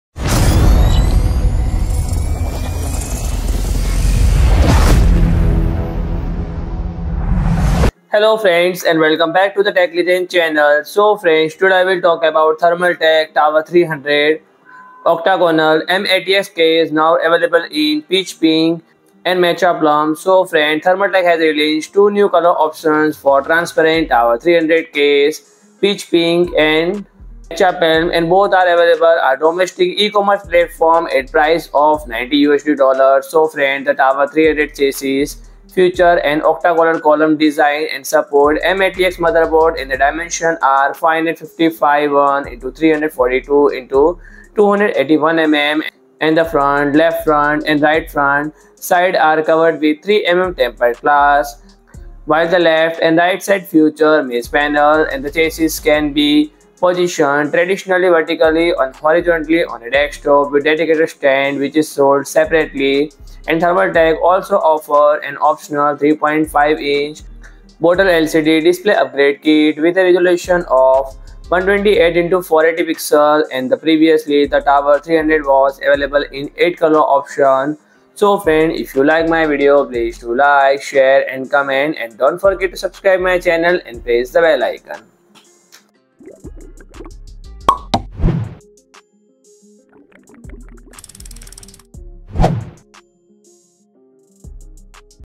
Thermaltake Tower 300 Octagonal M-ATX Case Launched With Peach Pink and Matcha Plum Color
PRODUCT BRAND STORE AMAZON

Thermaltake has released two new color variants for its Transparent Tower 300 case: Peach Pink and Matcha Plum. Both are available on domestic e-commerce platforms at a price of $90.The Tower 300 chassis features an octagonal column design and supports M-ATX motherboards. Its dimensions are 551×342×281 mm. The front, left front, and right front sides are covered with 3mm tempered glass, while the left and right sides feature mesh panels. This chassis can be positioned traditionally vertically or horizontally on a desktop with a dedicated stand, which is sold separately. Thermaltake also offers an optional 3.5-inch bottom LCD display upgrade kit with a resolution of 128×480 pixels. Previously, the Tower 300 was available in eight colors: black, white, hydrangea blue, matcha green, racing green, turquoise green, honeycomb yellow, and bubble pink. The new Peach Pink and Matcha Plum variants provide additional options for users. The Tower 300 employs a chimney effect cooling solution to enhance vertical air convection, improving heat dissipation.